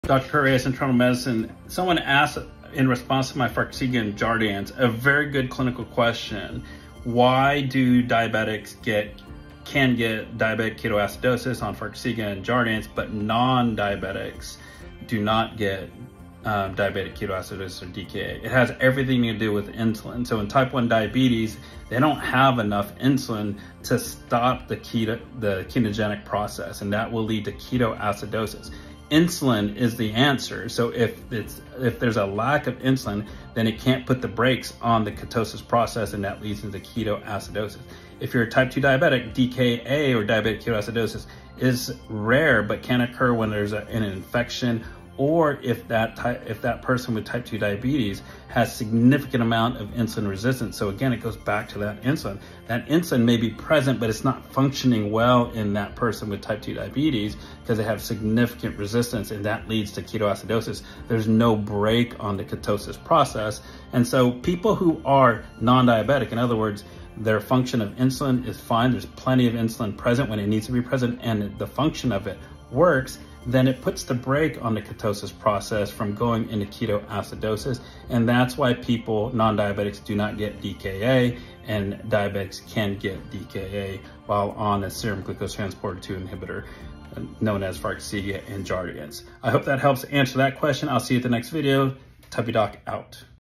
Understanding Diabetic Ketoacidosis with Farxiga and Jardiance
Why do diabetics risk DKA with Farxiga and Jardiance while non-diabetics don't? 🤔 Insulin plays a crucial role! Dive into the science with me, Tubby Doc.